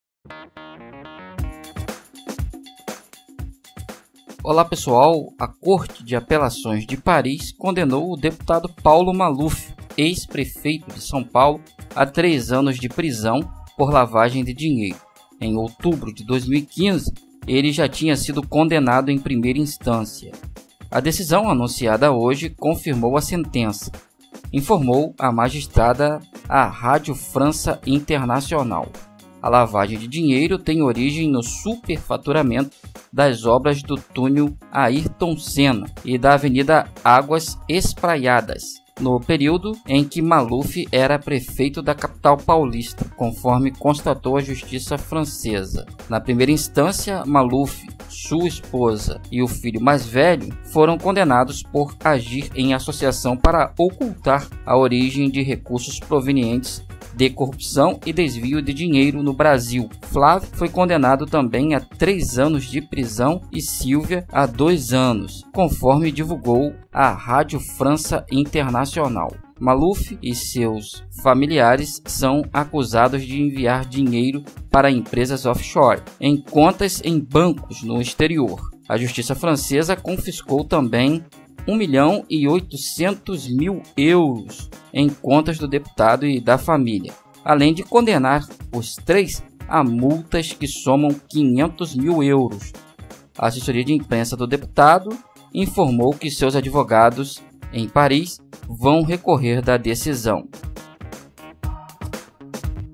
Justiça francesa confirma sentença de Maluf.
A Corte de Apelações de Paris condenou o deputado Paulo Maluf (PP), ex-prefeito de São Paulo, a três anos de prisão por lavagem de dinheiro.  A decisão anunciada hoje (20) confirmou a sentença, informou a magistrada da Corte Magali Josse à Rádio França Internacional (RFI).